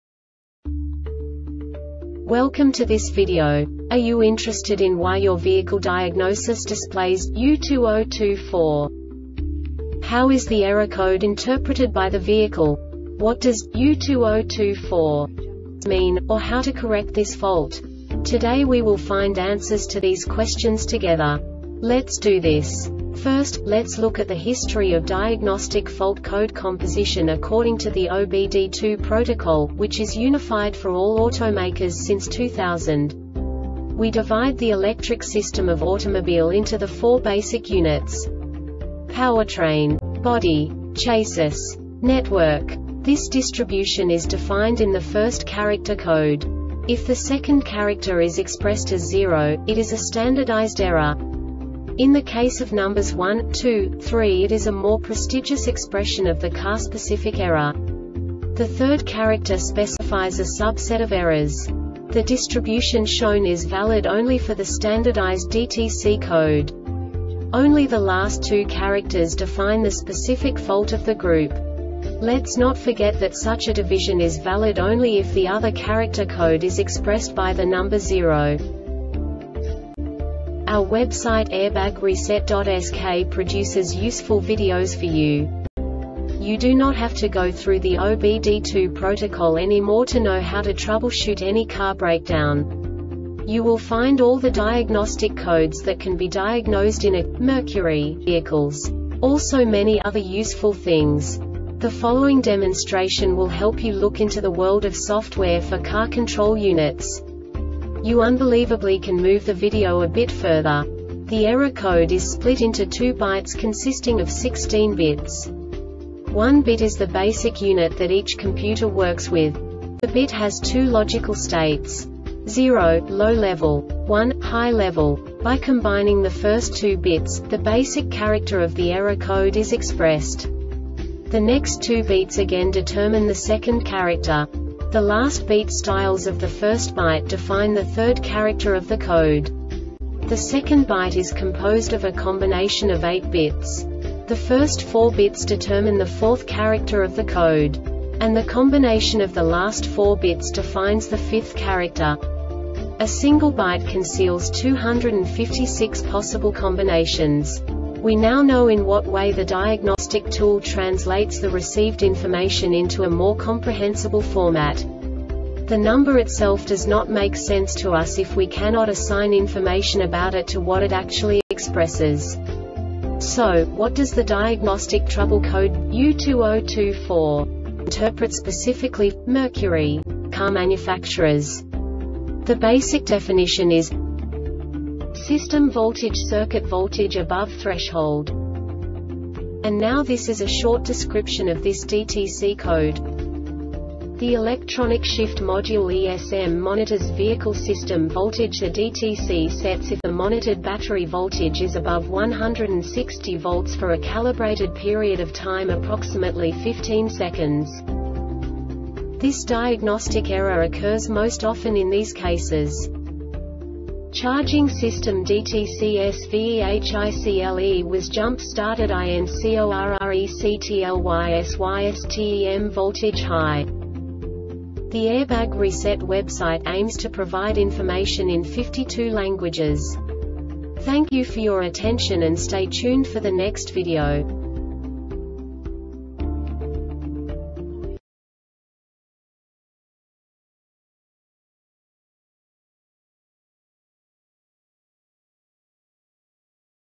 DTC Mercury U2024 Short Explanation
Contents:
0:21 Basic DTC analysis according to OBD2 protocol standard.
1:48 Insight into programming
2:58 A diagnostically understandable description of the Diagnostic Trouble Code
3:05 Basic error definition

Make:
Mercury
Code:
U2024
Definition:
SYSTEM VOLTAGE-CIRCUIT VOLTAGE ABOVE THRESHOLD
Description:
The Electronic Shift Module (ESM) monitors vehicle system voltage. The DTC sets if the monitored battery voltage is above 16.0 volts for a calibrated period of time (approximately 15 seconds).
Cause:
CHARGING SYSTEM DTCSVEHICLE WAS JUMP STARTED INCORRECTLYSYSTEM VOLTAGE HIGH